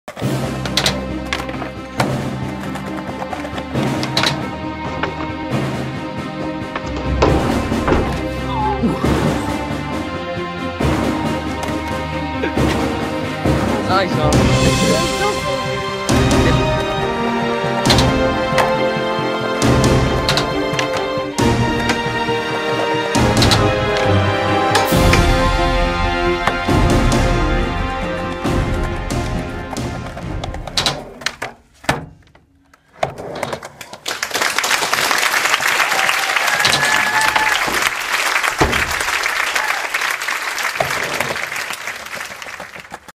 Young skater lands Blunt stall to Rock to fakie for the first time!
» INSTAGRAM: @SkateandThrash1 @Ferdehh @Martyallday @Marijnnstolk @Hen3Luigi